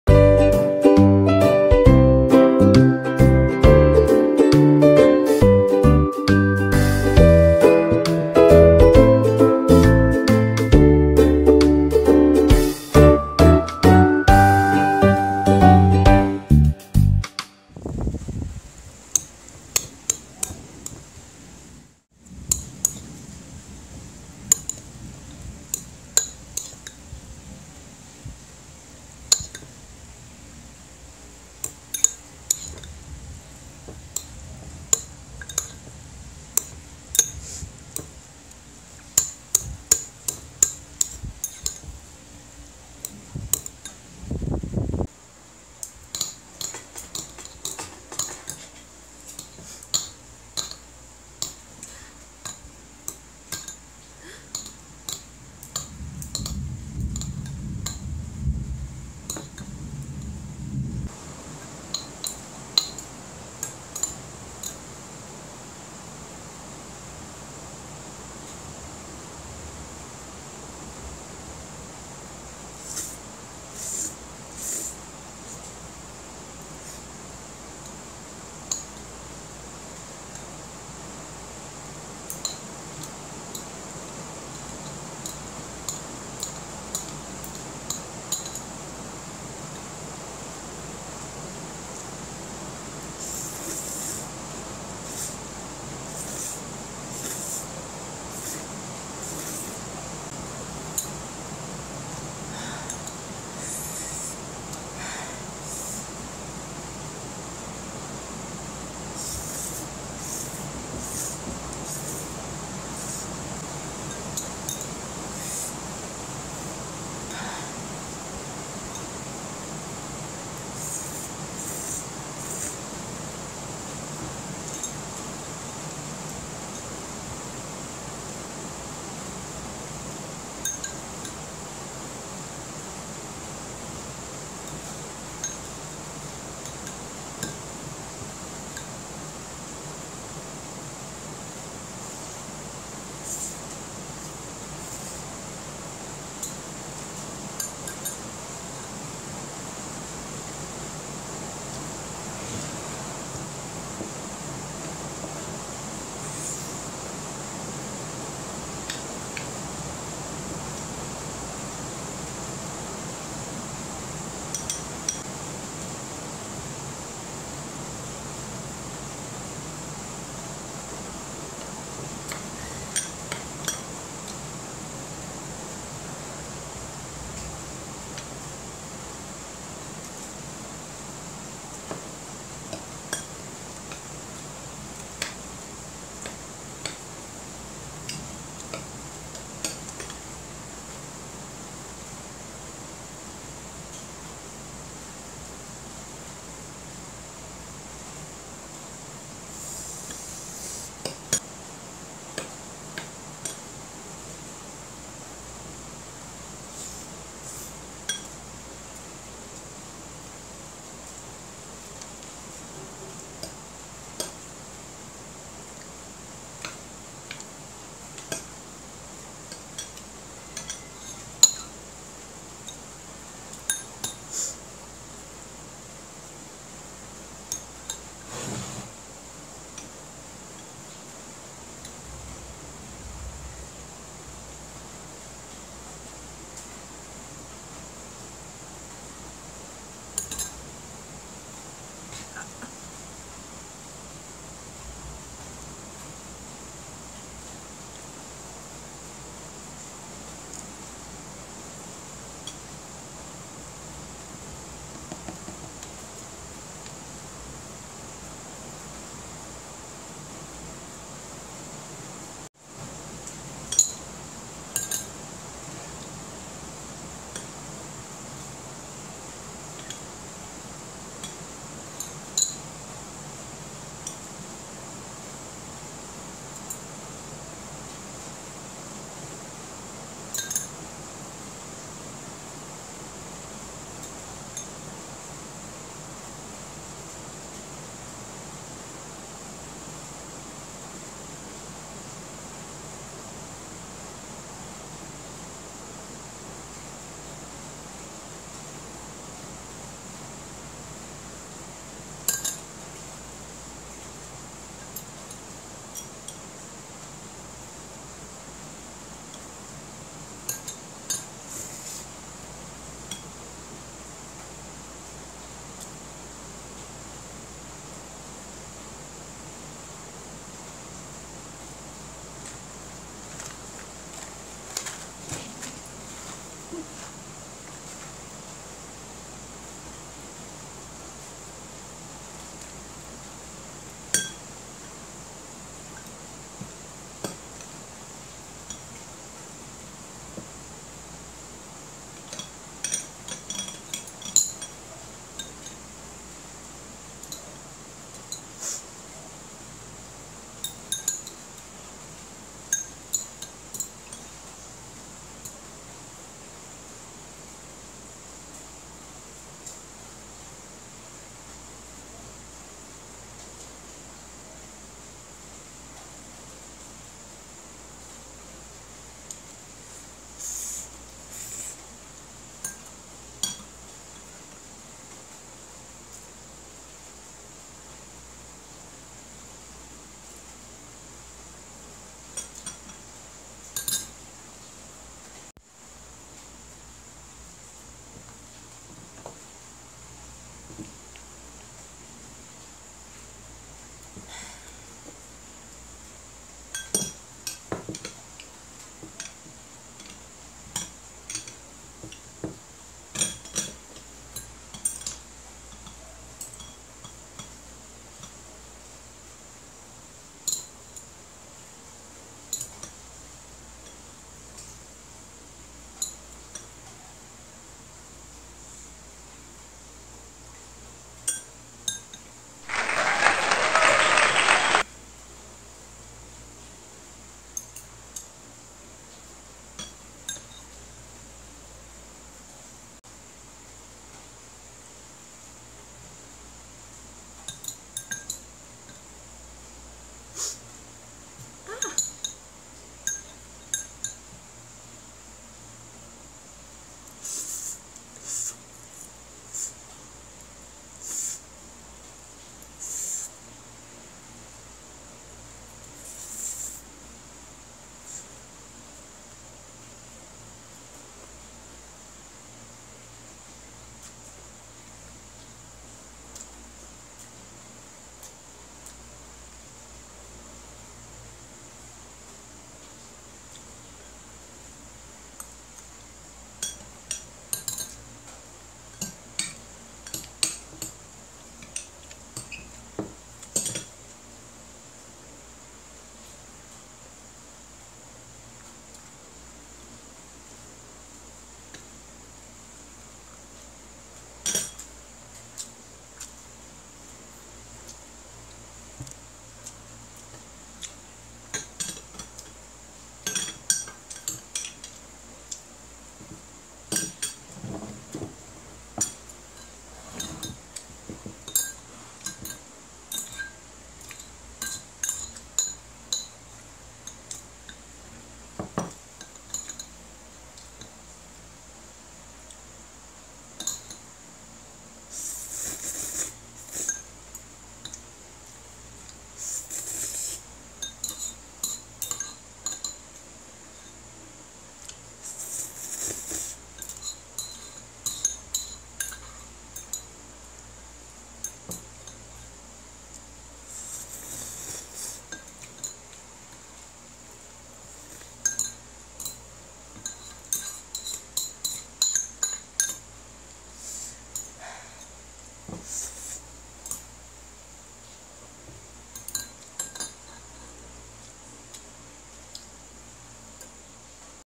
Eating Bibimyeon (Cold Noodles) | Magic Homestaurant
I had bibimyeon with pork and rice
Once again I was joined by my cat
No music eating show

URL: InShot